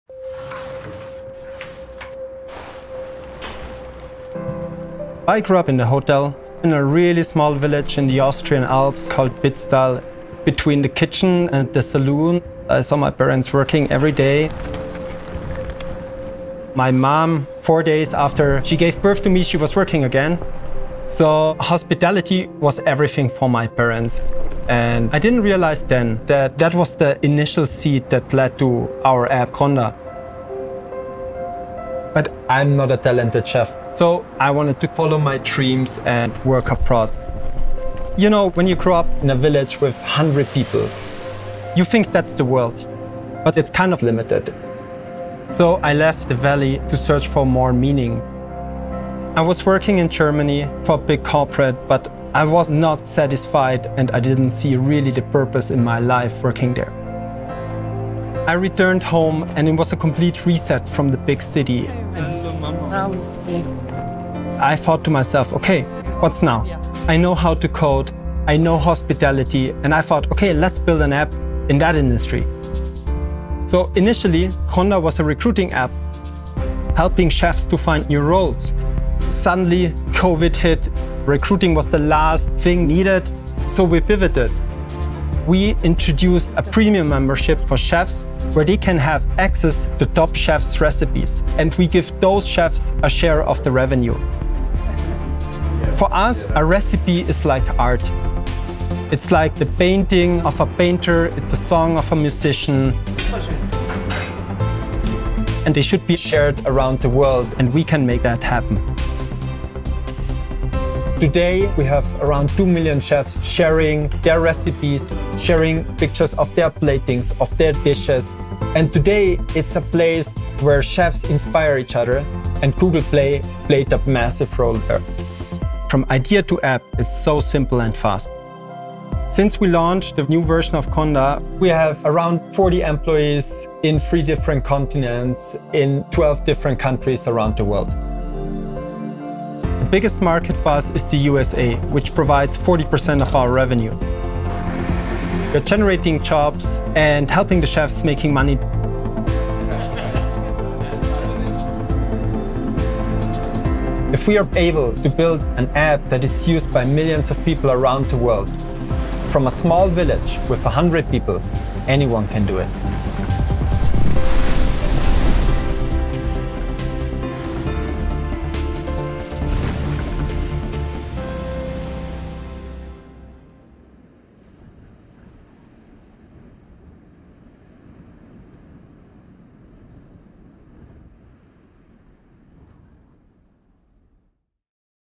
WeArePlay | Valentin | Gronda | Austria
Meet Valentin from Innsbruck, Austria. He co-founded Gronda, a place where chefs can share their recipes and bring the world’s culinary knowledge into the pocket of foodies everywhere.

WeArePlay celebrates the global community of people creating apps and games businesses on Google Play.